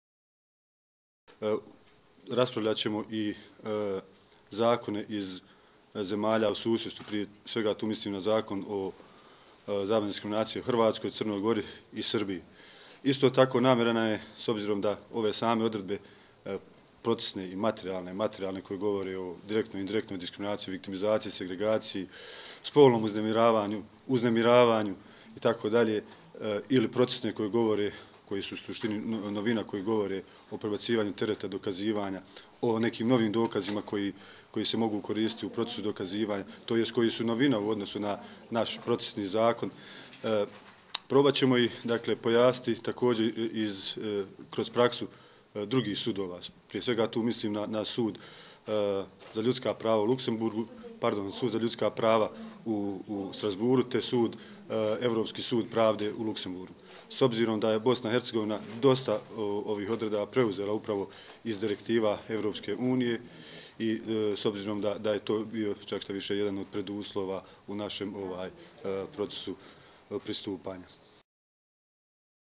Hajro Poskovic, OSCE - on Anti-discrimination Law in BiH, Statement
ENG - Sarajevo, Hajro Poskovic, Legal Advisor in OSCE Mission to Bosnia and Herzegovina reflects on Anti-discrimination Law in BiH
LOC - Sarajevo, Hajro Pošković, Pravni savjetnik u Misiji OSCE-a u BiH govori o Zakonu o zabrani diskriminacije u BiH